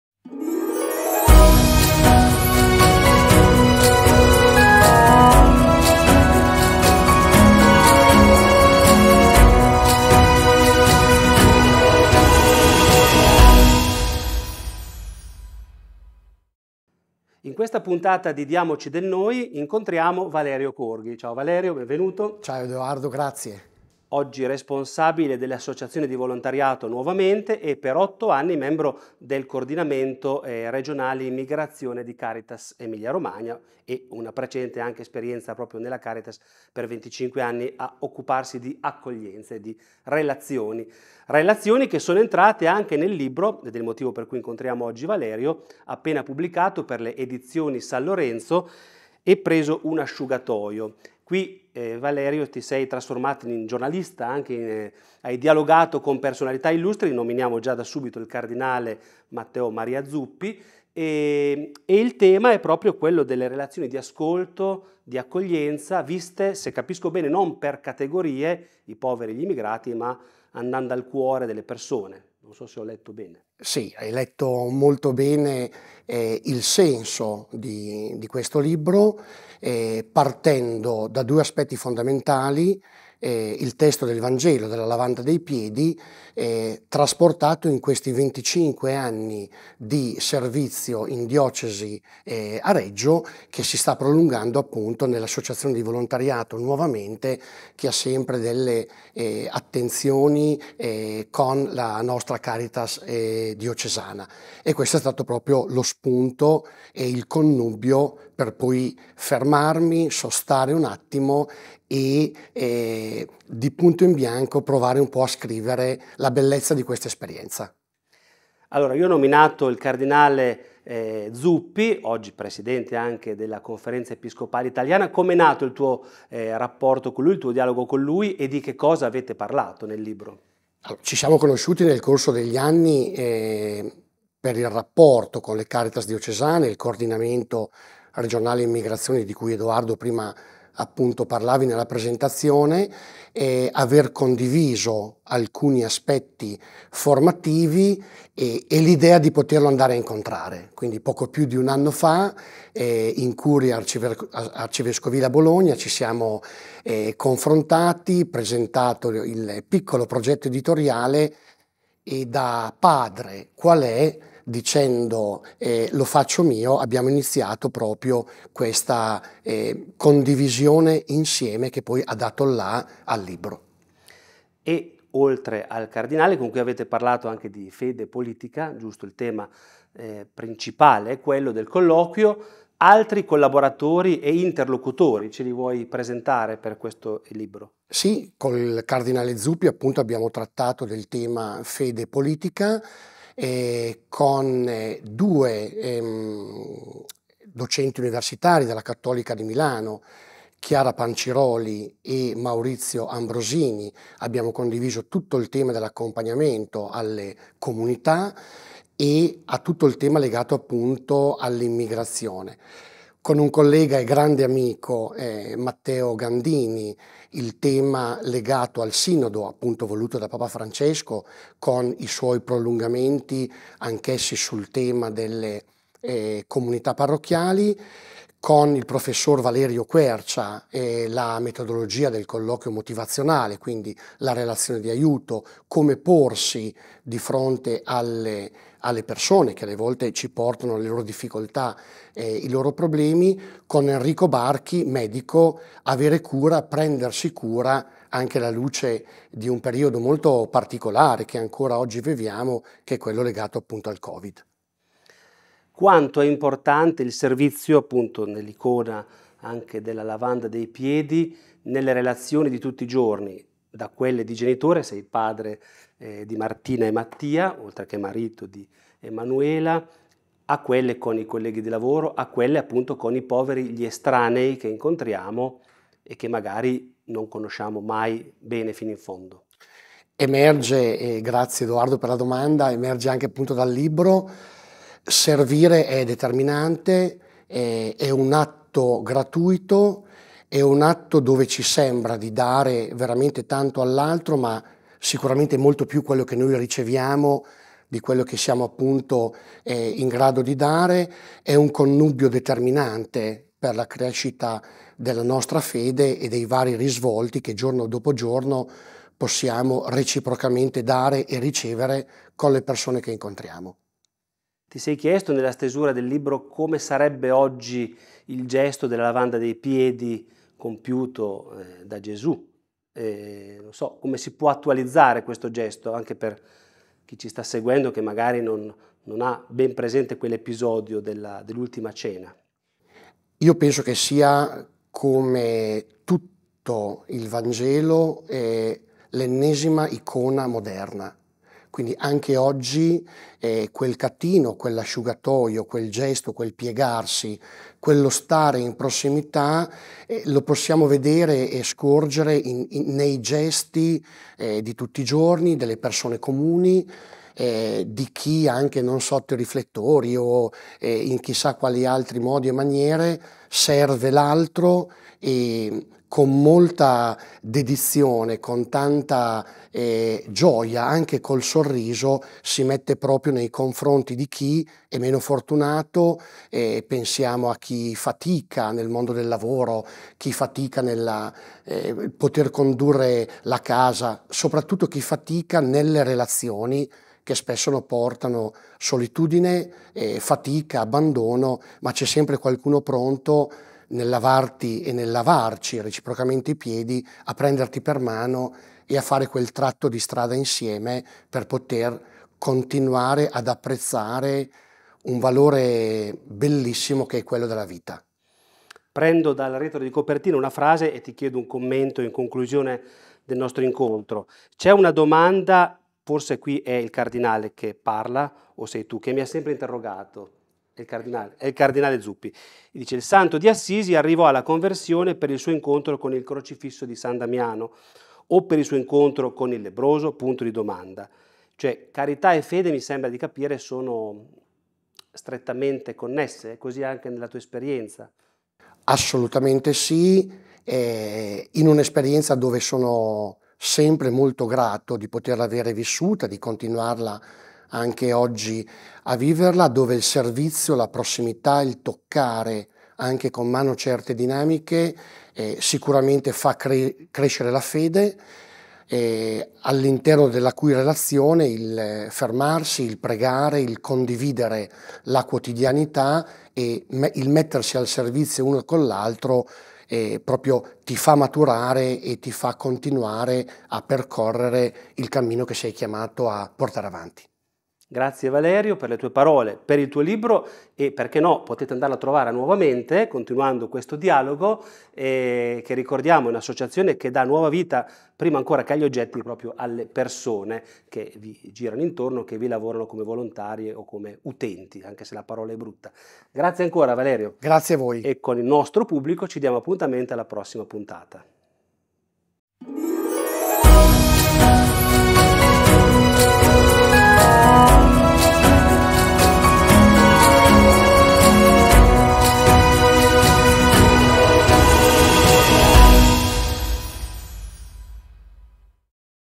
E preso un asciugatoio... Valerio Corghi
Valerio Corghi, coordinatore di Nuovamente e prima ancora della rete di accoglienza della Caritas diocesana, ha pubblicato il libro "E preso un asciugatoio...". Nel volume l'autore riflette in particolare su relazione e servizio dialogando con altri ospiti,

00:00 - Diamoci del NOI
00:16 - Relazioni nate nel servizio
01:58 - Il cardinale Matteo Maria Zuppi
02:54 - Un libro dialogico
04:16 - Il servizio nelle relazioni di tutti i giorni
05:14 - Lavanda dei piedi oggi
06:49 - Carità e fede
08:22 - Diamoci del NOI